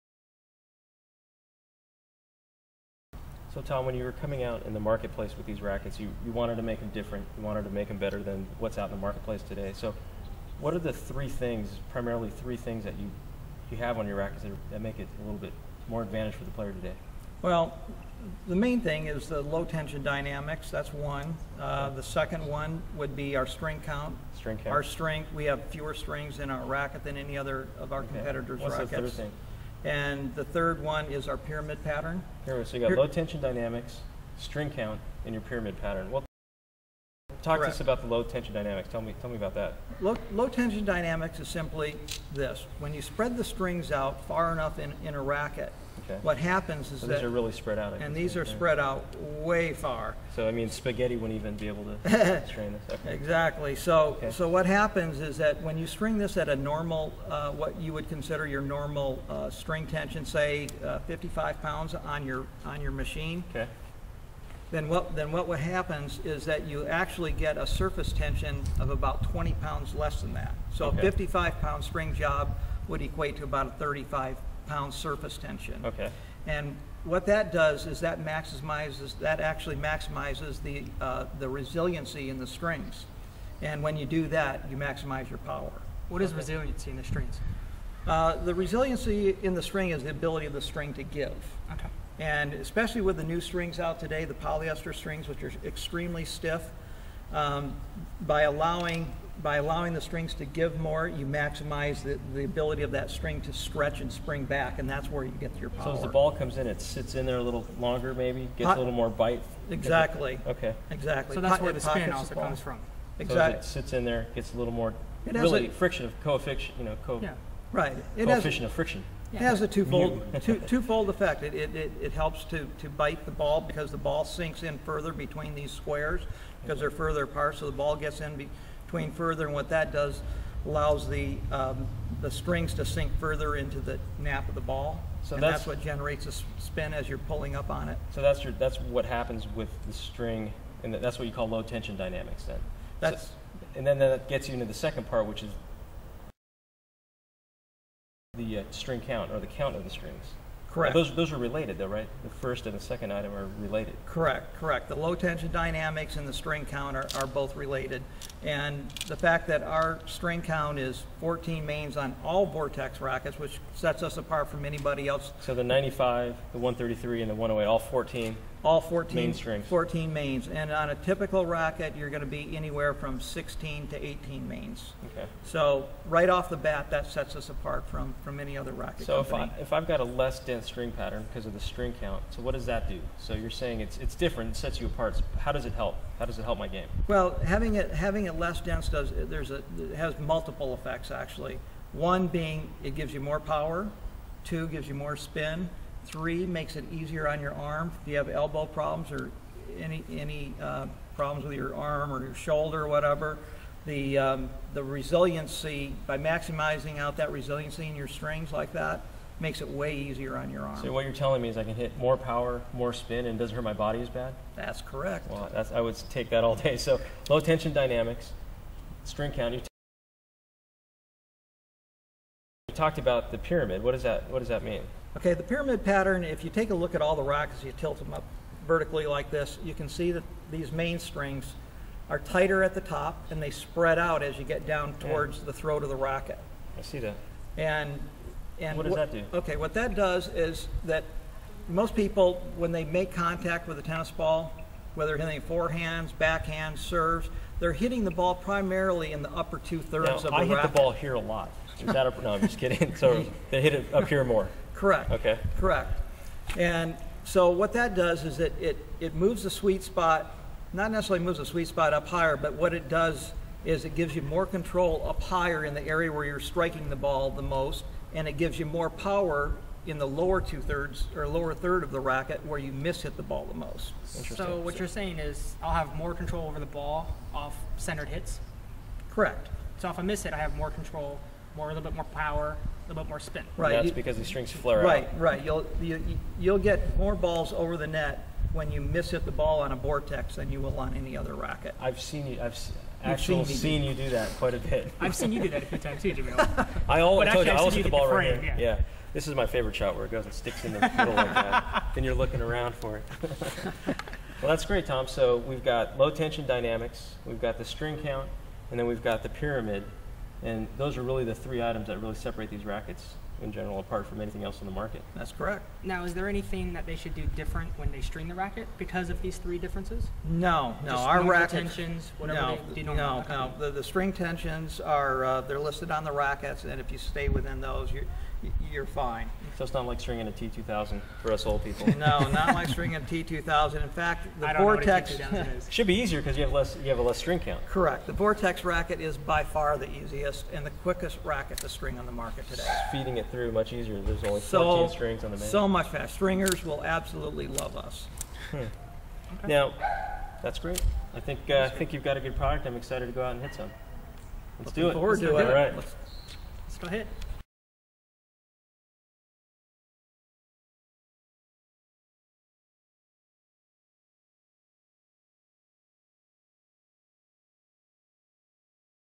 The Vortex Difference
The Vortex Difference.  Explains the differences of the Vortex Tennis racquets and their new innovations